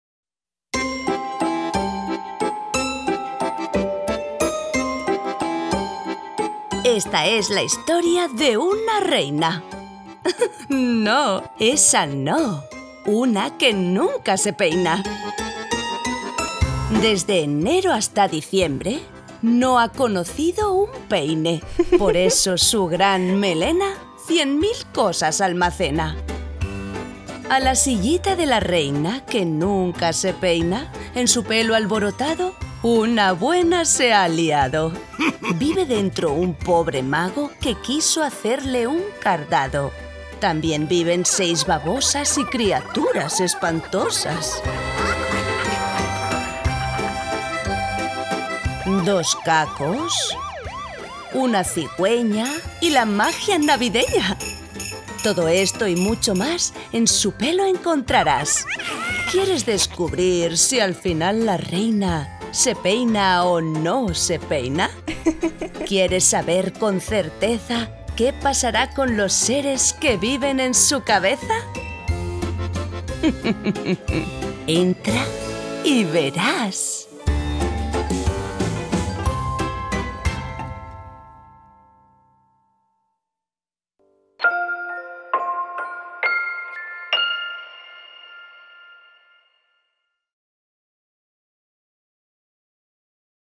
LA SILLITA DE LA REINA (QUE NUNCA SE PEINA) - BOOKTRAILER
Esta es la historia de una reina...
Noooo, esa noooo. ¡Una que nunca se peina! Desde enero hasta diciembre no ha conocido un peine. Por eso, su gran melena cien mil cosas almacena.
A la sillita de la reina que nunca se peina, en su pelo alborotado, una buena se ha liado.
Vive dentro un pobre mago que quiso hacerle un cardado. También viven seis babosas y criaturas espantosas. Dos cacos, una cigüeña y la magia navideña. Todo esto y muchos más en su pelo encontrarás.

¿Quieres descubrir si al final la reina se peina o no se peina? ¿Quieres saber con certeza qué pasará con los seres que viven en su cabeza?

Texto: Julia Pérez Villegas.
Ilustraciones: Desirée arancibia.
Editorial: Tu cuento y tú.